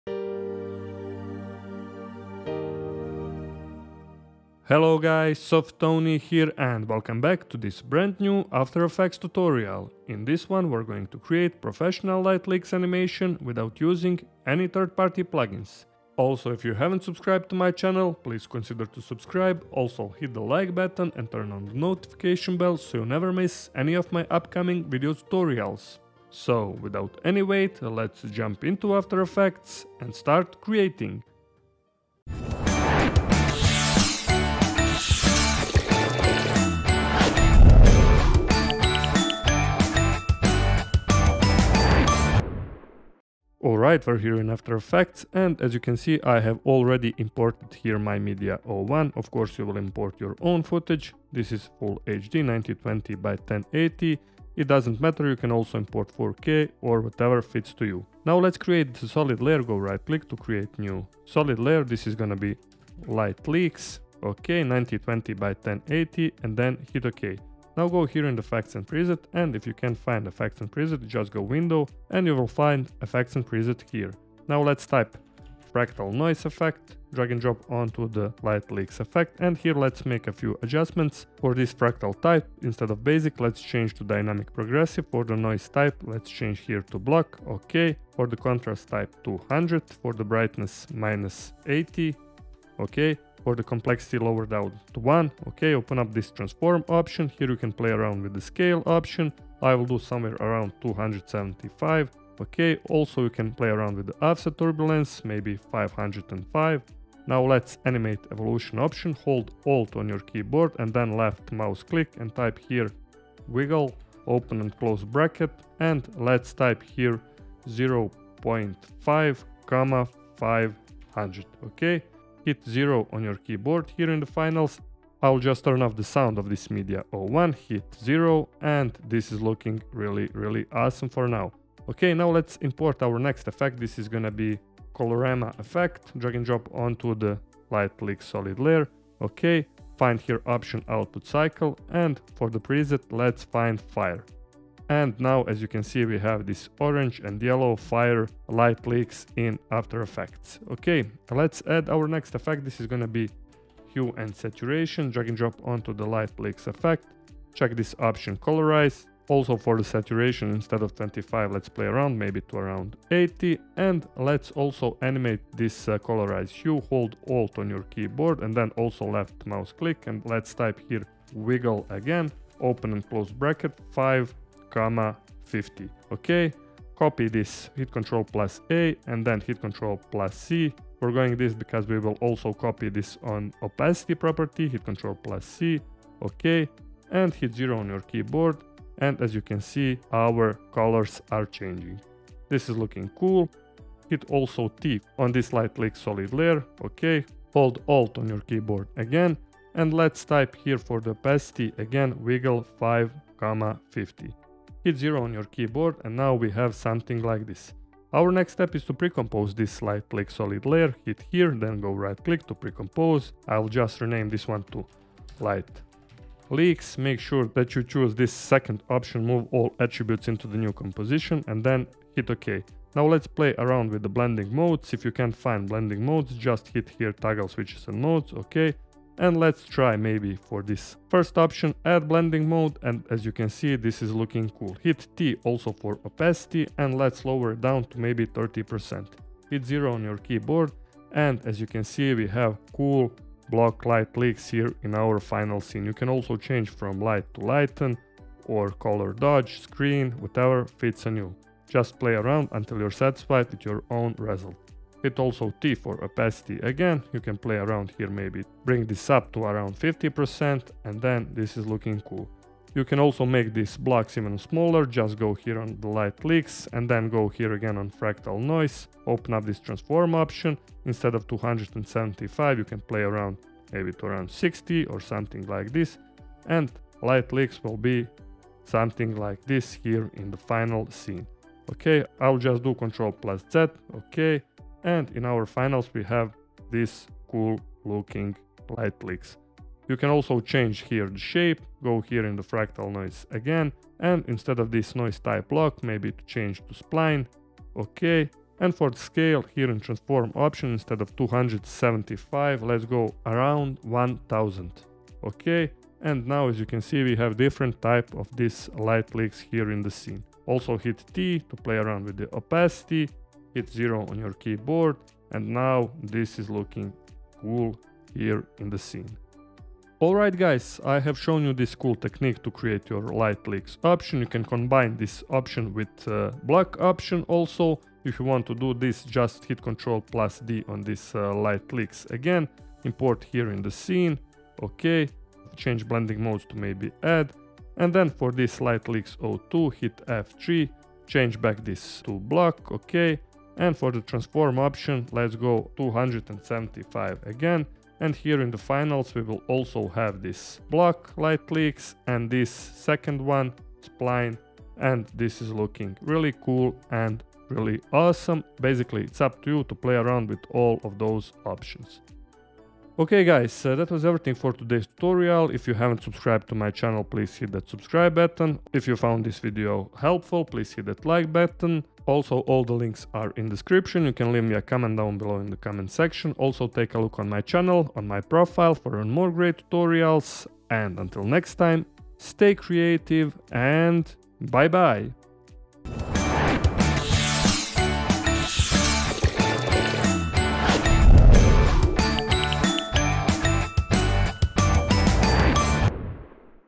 Create Professional LIGHT LEAKS in After Effects - After Effects Tutorial
Hey everyone, in this after effects tutorial I will show you how to create professional Light Leaks in After Effects without using any third-party plugins. This technique is a cool way to make your videos or images looking even better and to impress your audience. Very easy to make, just follow my steps!

*** My Portfolio - Stock Sites ***

*** Do you like music - Check out Senchi's portfolio ***